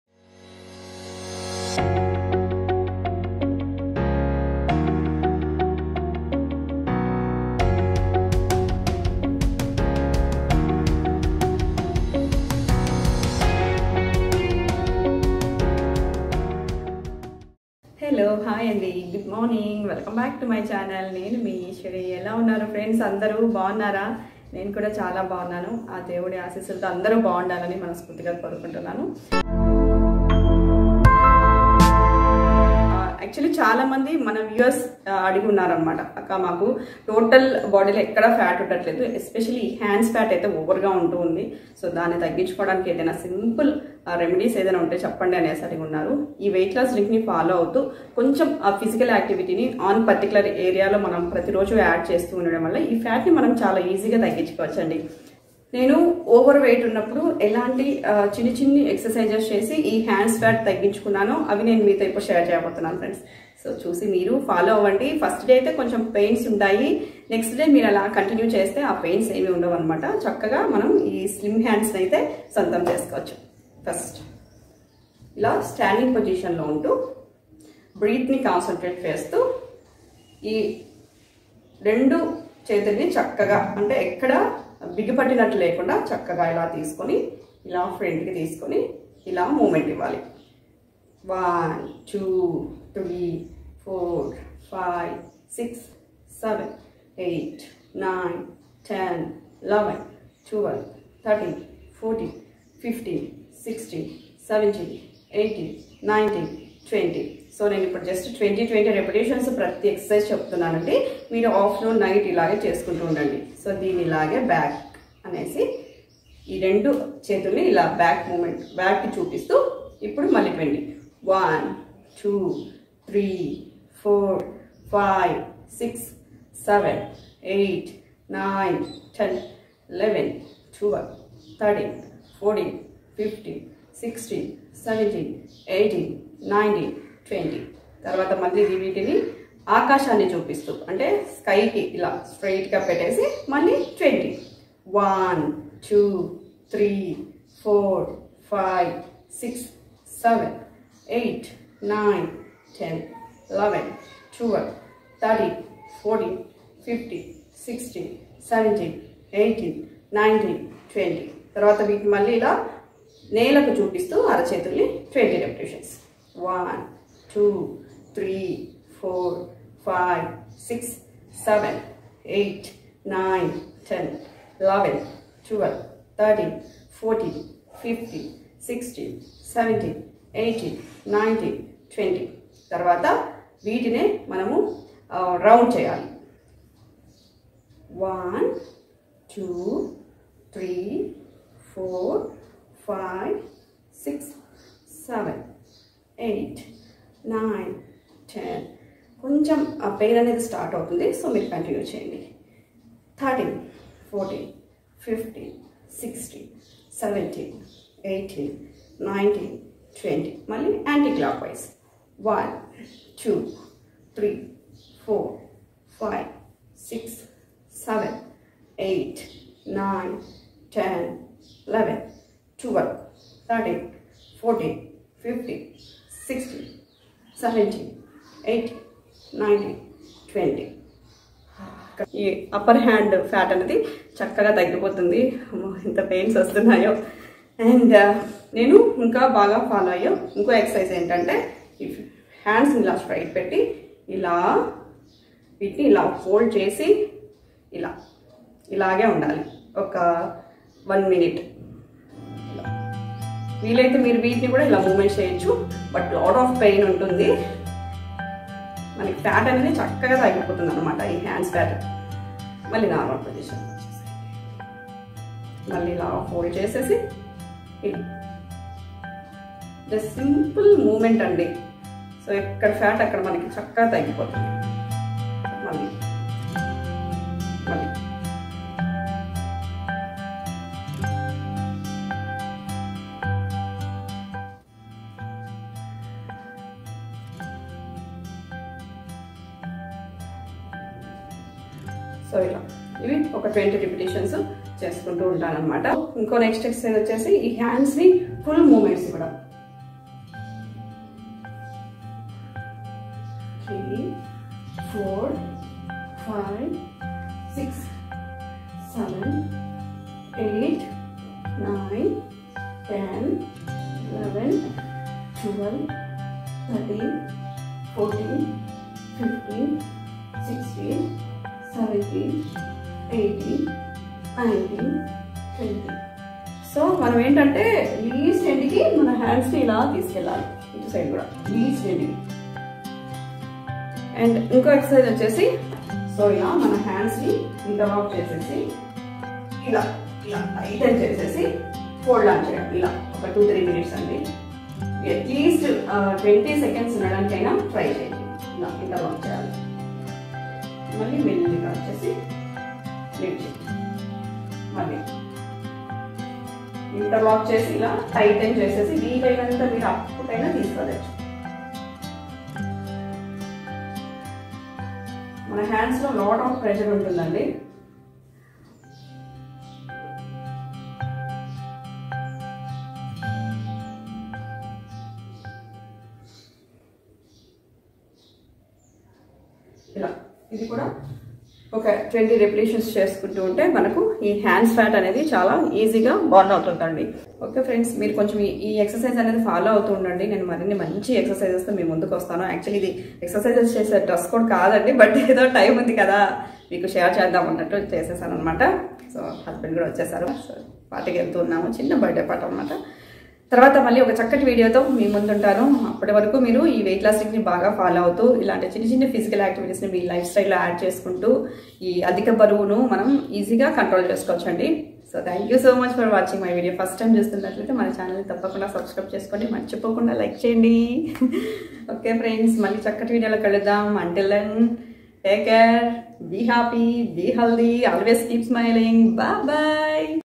💪Hands fatkiller exercisesచేతులు లావుగా కనిపిస్తున్నాయా?ఈ కొన్ని exercises చాలు🤷‍♀️1weekలోslim Hands
fast fat burning exercises for hands, first day pains ఉంటాయి hands,ఏమీ కాదు అలానే రోజూ exercises చేస్తూ ఉంటే చాల త్వరగా చేతుల్లో కొవ్వు కరుగుతుంది